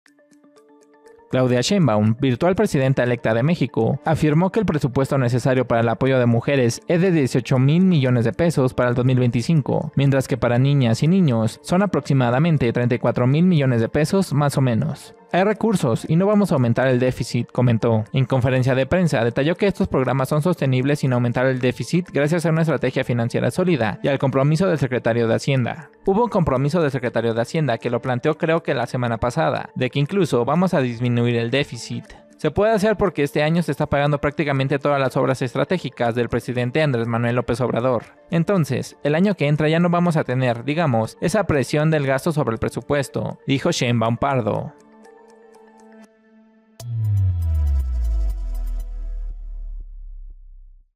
Claudia Sheinbaum comenta que no aumentará el déficit por programas sociales
La virtual triunfadora del proceso electoral de este año consideró que hay oportunidades de aumentar la recaudación sin una reforma fiscal profunda.